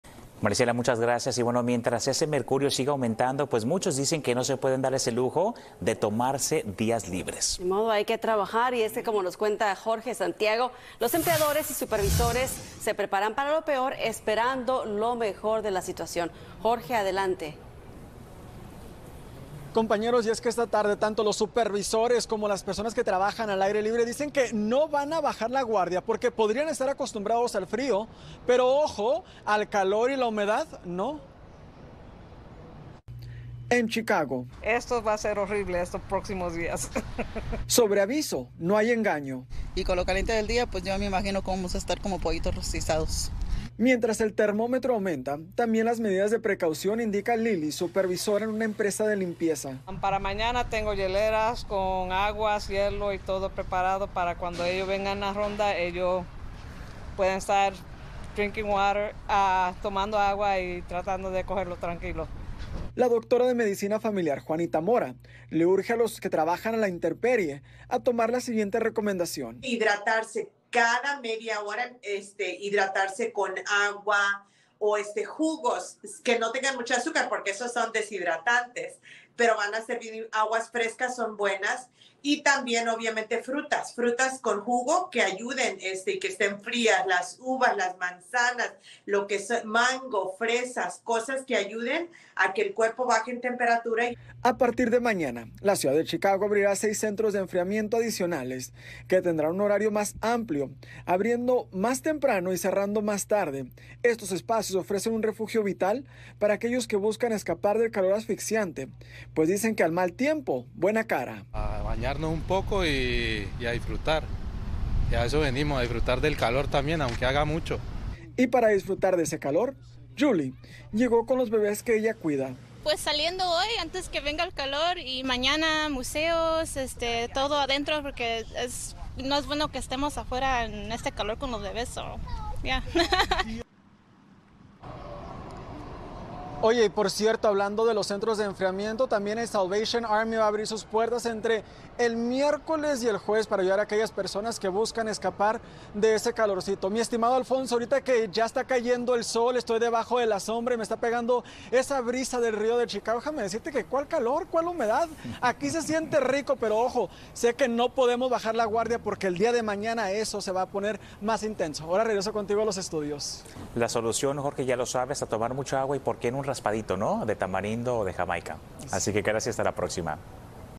¿Qué puedo consumir para combatir el calor?
Expertos comparten recomendaciones de alimentos y bebidas que la gente puede consumir para refrescarse de las altas temperaturas y mantenerse hidratado.